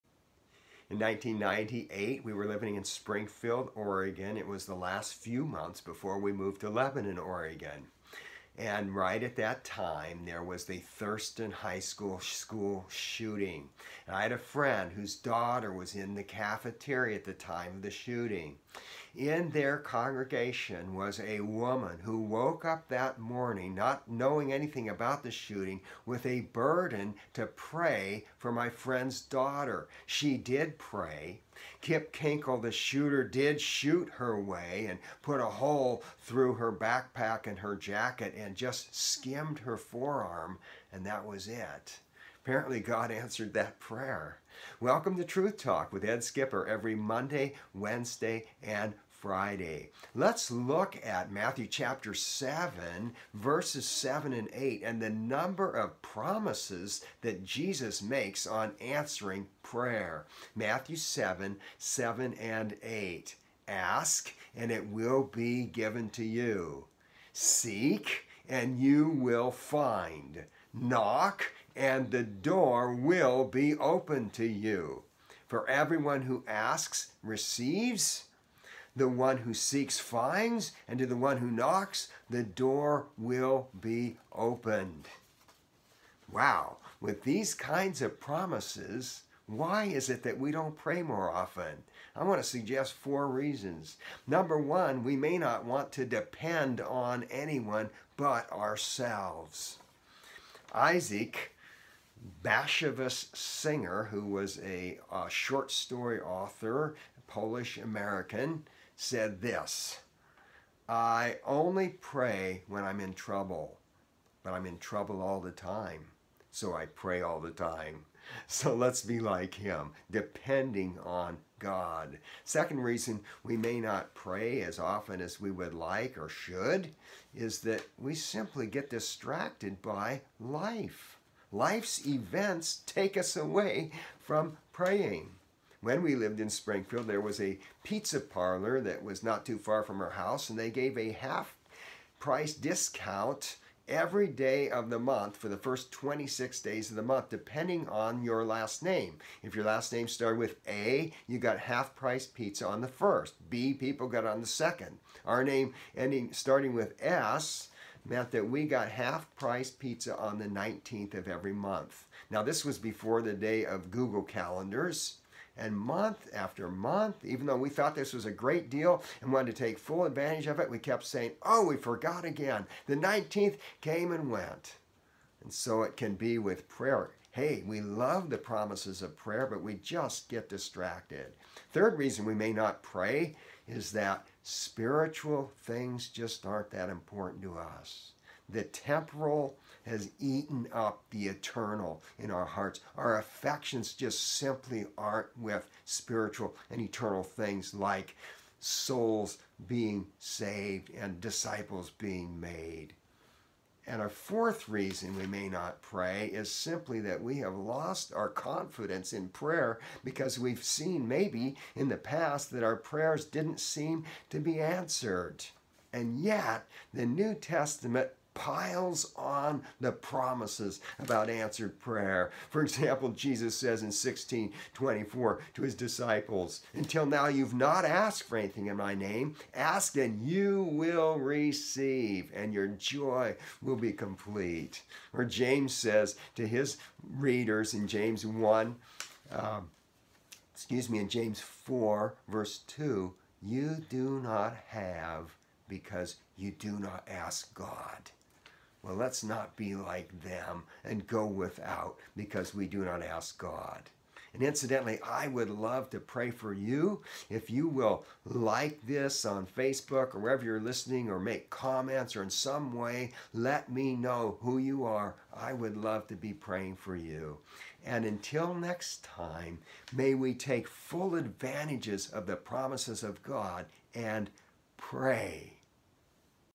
Why Not Pray? | TruthTalk with Ed Skipper | Episode 45
Scripture makes outstanding promises regarding prayer